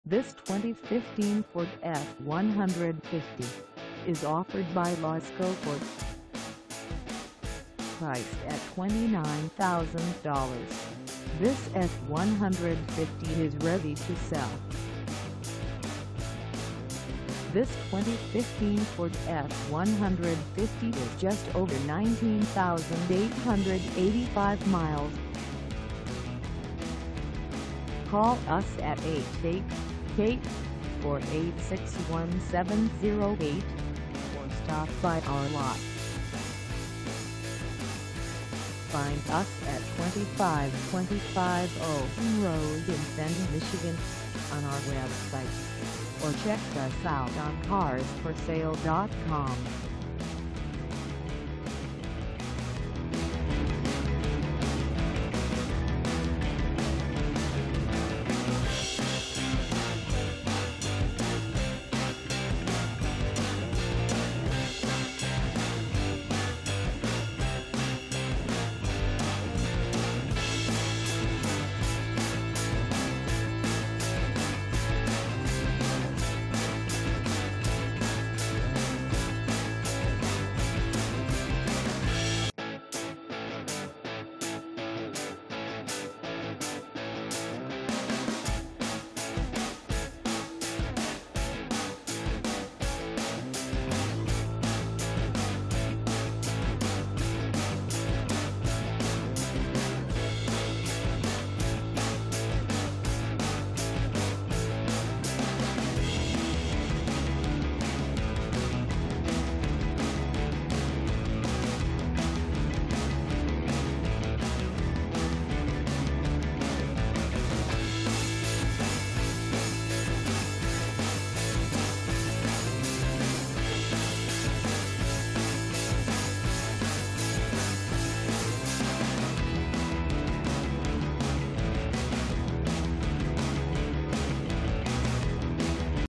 2015 Ford F-150 XL 4x4 4dr SuperCab 6.5 ft. SB for sale in F
This 2015 Ford F-150 XL 4x4 4dr SuperCab 6.5 ft. SB is for sale in FENTON, MI 48430 at LASCO FORD.

Engine: 2.7L V6
Transmission: Automatic 6-Speed
Color: Tuxedo Black Metallic
Mileage: 19885
Contact LASCO FORD 2525 OWEN RD FENTON, MI 48430 to test drive this 2015 Ford F-150 XL 4x4 4dr SuperCab 6.5 ft. SB today.